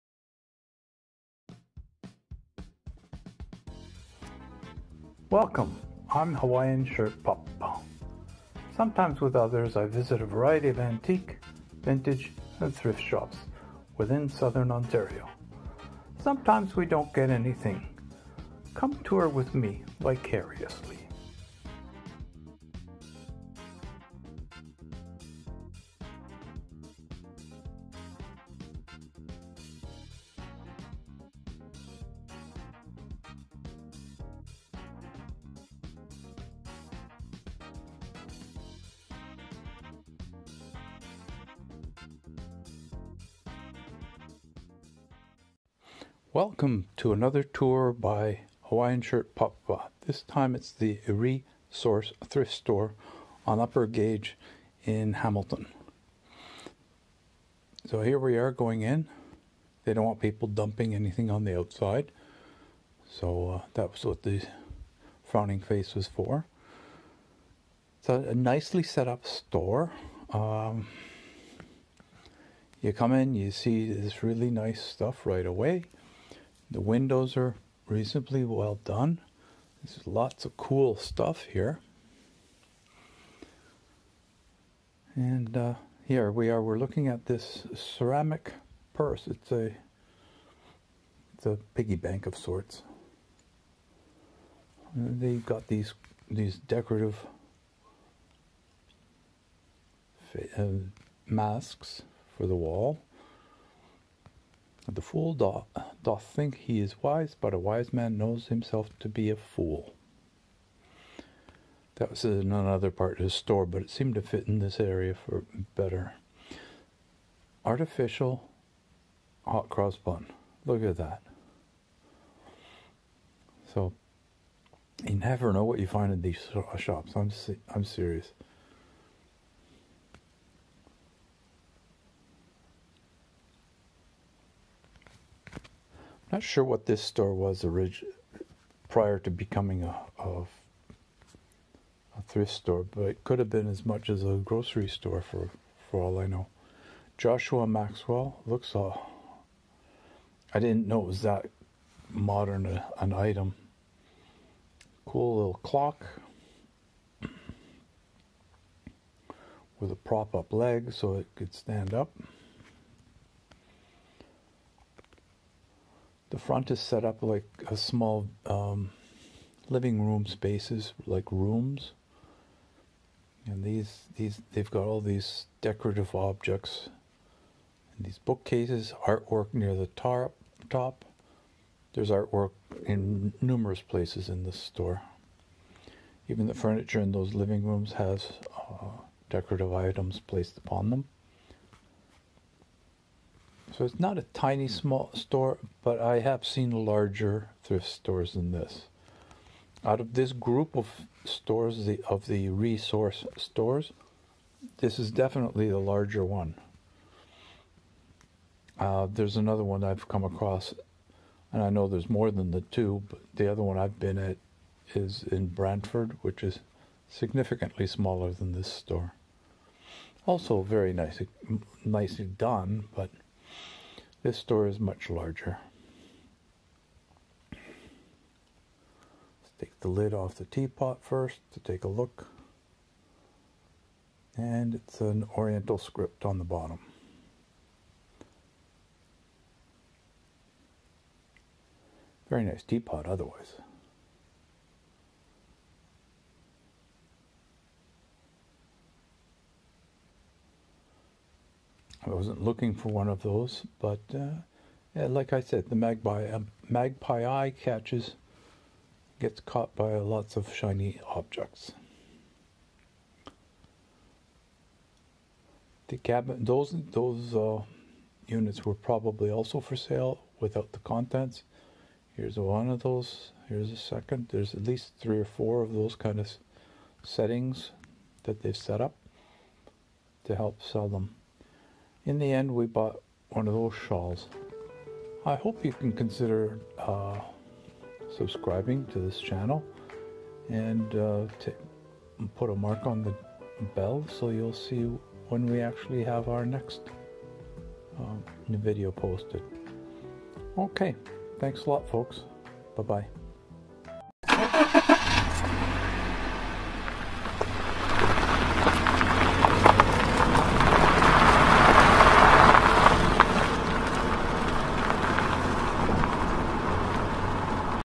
"Re-Source Thrift Store" Hamilton
Tour of the Re-Source Thrift Store on Upper Gage, near the Linc in Hamilton "mountain".
It's located at the south end of a strip plaza. There is a couple of other interesting places in the same plaza including a well loved Fish and Chip place and a new/used sporting goods store. the Mission Thrift is a few blocks north of this store.